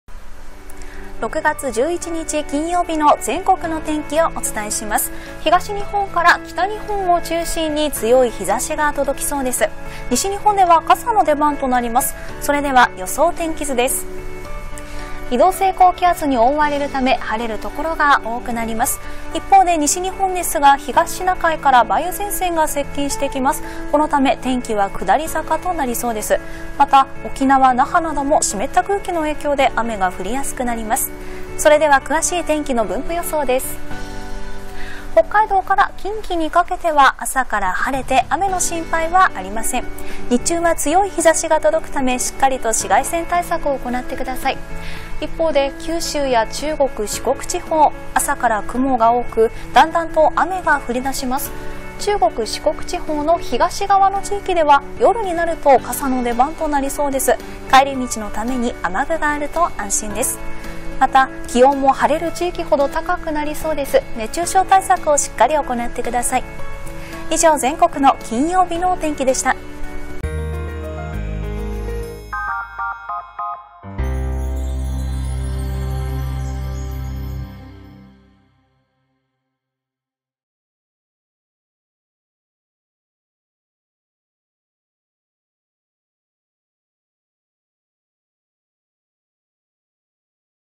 【今日の天気】西日本は天気下り坂 関東以北は晴れて暑さ続く
高気圧が段々と東に離れ、西日本で天気が崩れてきます。九州など早い所は昼頃から雨が降り出す見込みです。関東以北は晴れて暑さが続きます。